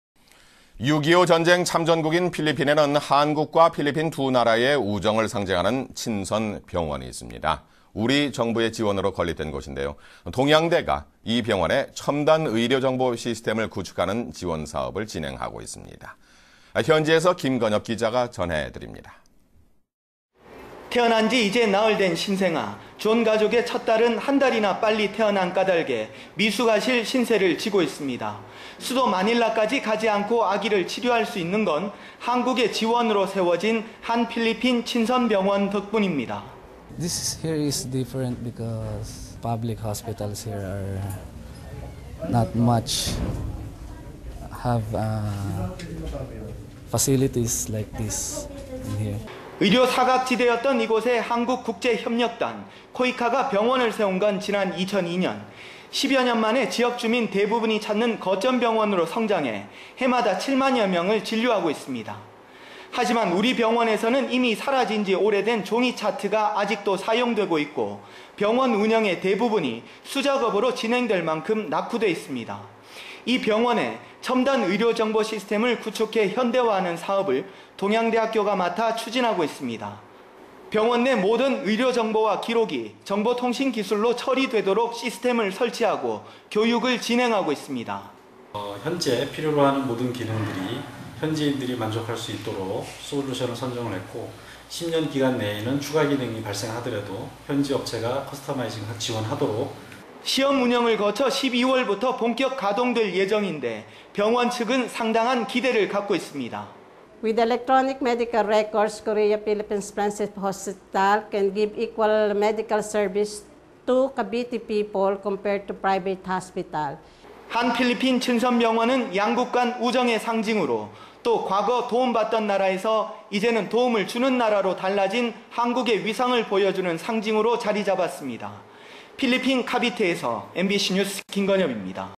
[안동MBC뉴스]한-필 친선병원 현대화
2013/09/25 15:28:36    작성자 : 보도팀장

           ◀ＡＮＣ▶
 6.25 참전국인 필리핀에는 한국과 필리핀
두나라의 우정을 상징하는 친선병원이 있습니다

 우리정부의 지원으로 건립된 곳인데요,
동양대가 이 병원에 첨단 의료정보시스템을
구축하는 지원사업을 진행하고 있습니다.

 김건엽기자가 현지에서 전해드립니다.
           ◀ＥＮＤ▶

 태어난지 이제 나흘된 신생아.

존 가족의 첫딸은 한달이나 빨리 태어난
까닭에 미숙아실 신세를 지고 있습니다.

  수도 마닐라까지 가지 않고 아기를
치료할 수 있는 건 한국의 지원으로 세워진
한-필리핀 친선병원 덕분입니다.

 ◀ＩＮＴ▶ 존/미숙아 보호자
 "이 병원은 달라요.주변 다른 병원들에는
  없는 의료시설을 갖고 있어요."

 의료 사각지대였던 이곳에 한국국제협락단
코이카가 병원을 세운 건 지난 2002년.

 십여년만에 지역 주민 대부분이 찾는
거점병원으로 성장해 해마다 7만여 명을
진료하고 있습니다.

 하지만 우리 병원에서는 이미 사라진지
오래된 종이차트가 아직도 사용되고 있고
병원운영의 대부분이 수작업으로 진행될만큼
낙후돼 있습니다.

 이 병원에 첨단 의료정보시스템을 구축해
현대화하는 사업을 동양대학교가 맡아
추진하고 있습니다.

 병원내 모든 의료정보와 기록이
정보통신기술로 처리되도록 시스템을 설치하고
교육을 진행하고 있습니다.

 ◀ＩＮＴ▶ 류연식 교수/동양대

 시험운영을 거쳐 12월부터 본격 가동될
예정인데 병원측은 상당한 기대를 갖고
있습니다.

 ◀ＩＮＴ▶ 프란시스코 병원장
"EMR시스템을 통해 한-필 친선병원은
 카비테 주민들에게 민간 사설병원과 동등한
 의료서비스를 제공할 수 있게 되었습니다."

 (S/U)한-필리핀 친선병원은
양국간 우정의 상징으로,또 과거 도움받던
나라에서 이제는 도움을 주는 나라로
달라진 한국의 위상을 보여주는 상징으로
자리잡았습니다.필리핀 카비테에서
MBC뉴스 김건엽입니다.